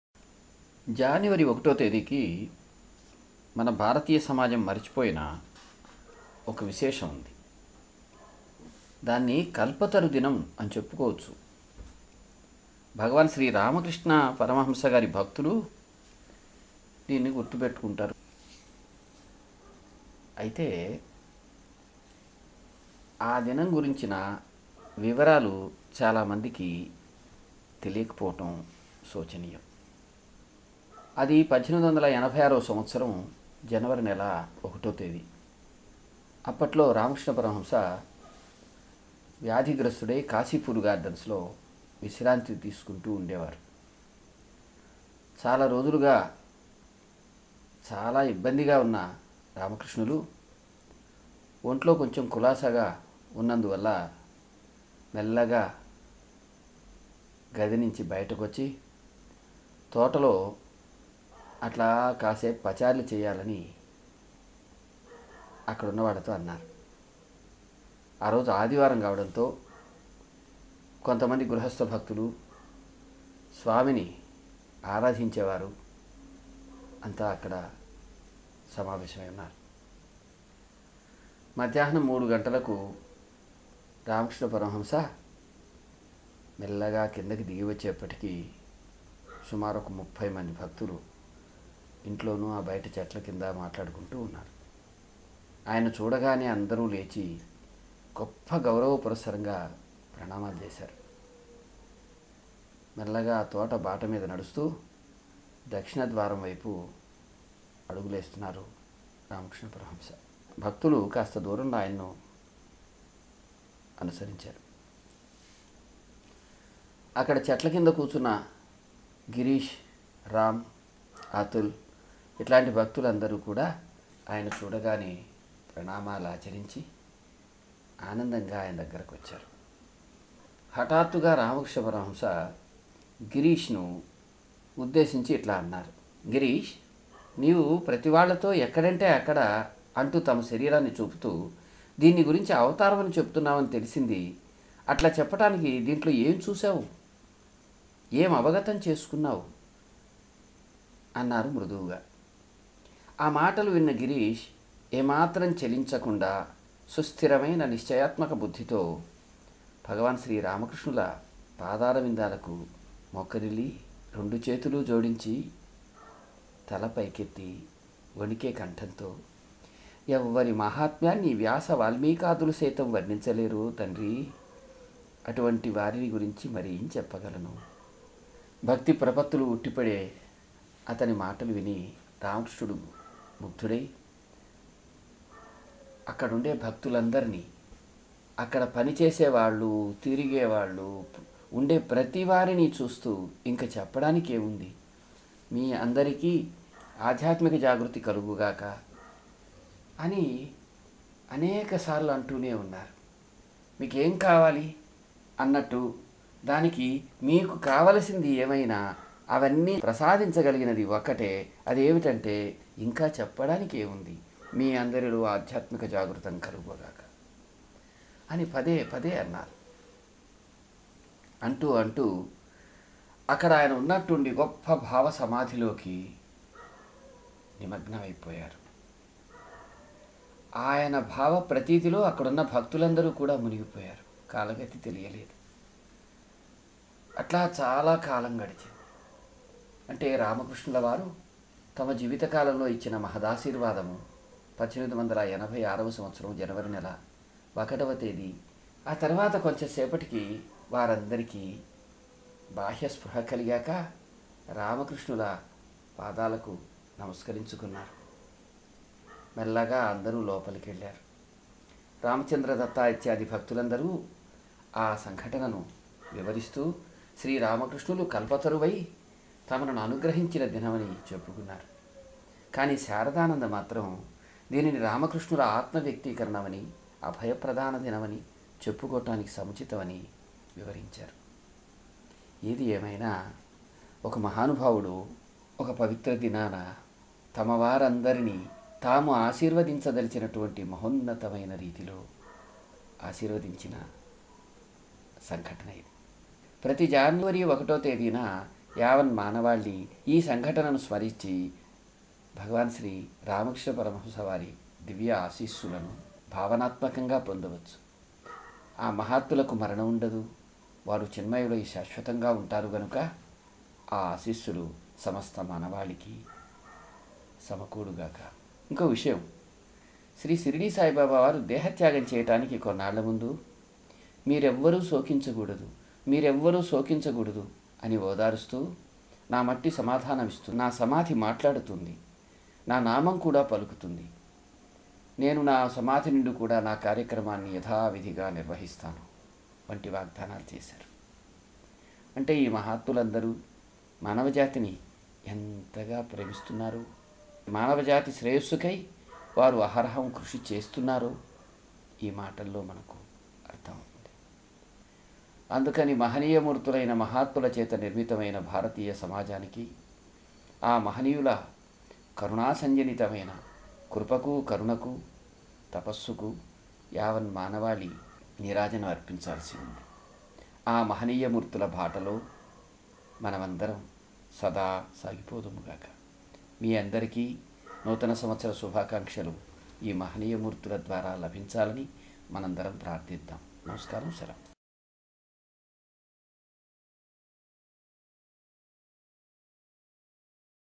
KALPATARU DAY, THE FIRST JANUARY
TO REMEMBER AM EVENT FORGETTEN BY THE PEOPLE OF INDIA.  SPECIALLY BLESSED BY SRI RAMAKRISHNA PARAMAHAMSA AT KASIPUR GARDENS,KOLKOTTA, IN 1886. HE BLESSED THE MANKIND ALONG WITH THE PEOPLE PRESENT.  SO HAVE THE BLESSINGS OF GREAT PARAMAHAMSA